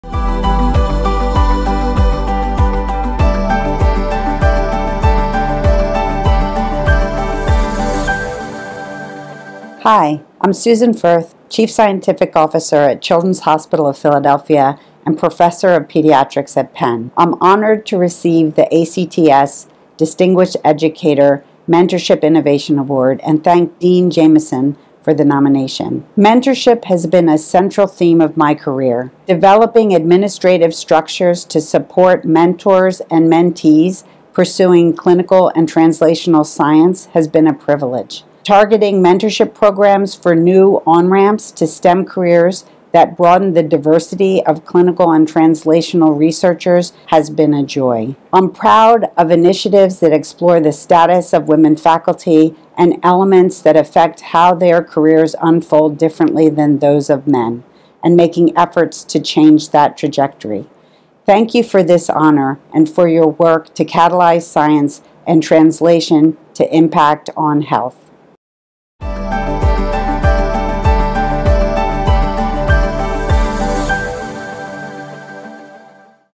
2023 ACTS Distinguished Educator Award: Mentorship Innovation: Susan Furth, MD PhD
The ACTS Distinguished Educator Award is presented to clinical and/or translational investigators who have distinguished themselves as an educator, mentor and leader, resulting in significant scientific accomplishments by the next generation of clinical and translational researchers. This award recognizes global innovation and impact in education and mentorship for the health sciences.

Specifically, the ACTS Distinguished Educator Award: Mentorship Innovation recognizes distinguished mentors with outstanding innovation in mentorship resulting in significant scientific accomplishments by the next generation of clinical and translational researchers and/or innovative approaches in mentorship to promote diversity and retention of under-represented groups in translational research.